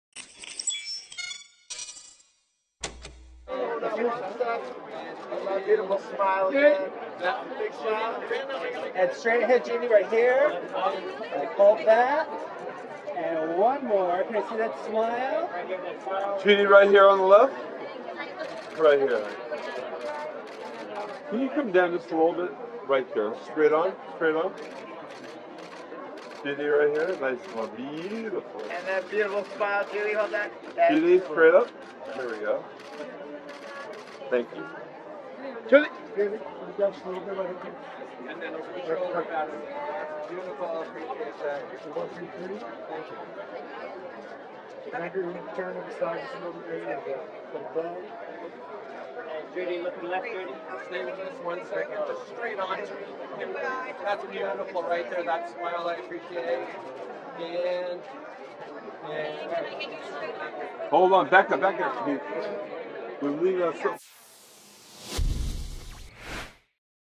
Judy Reyes "37th Annual Imagen Awards" Green Carpet Arrivals
Broll footage: Actress Judy Reyes on the green carpet at the 37th annual Imagen Awards held at LA Plaza de Cultura y Artes in Los Angeles, California USA on Sunday, October 2, 2022